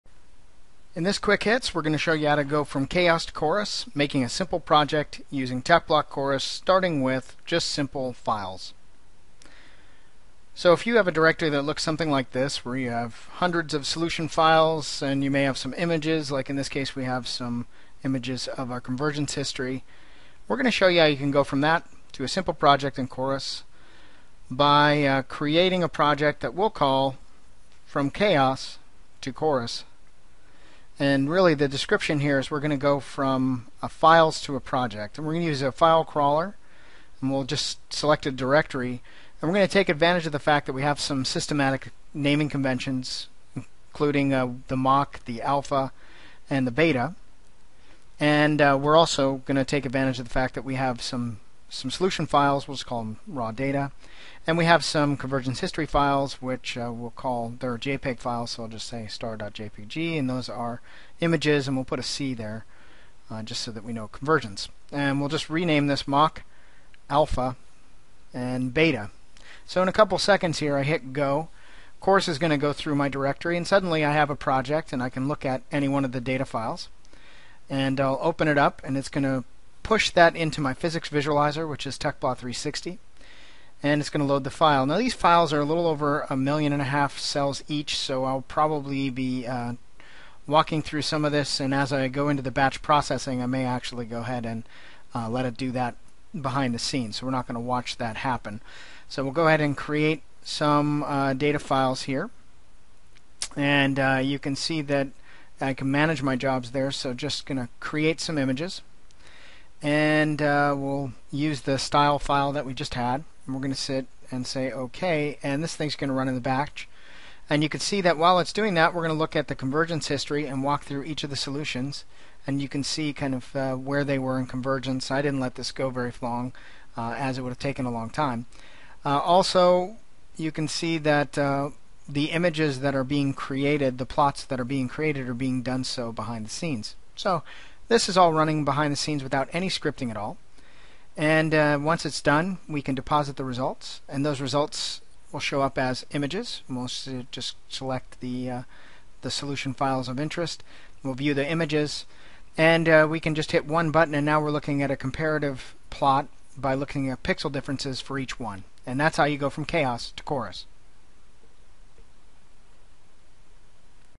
From Chaos to Chorus: Creating a project from 100 CFD results using Teplot Chours
In this example we will go from 100 simulation results and images to a project in 2:00 in Tecplot Chorus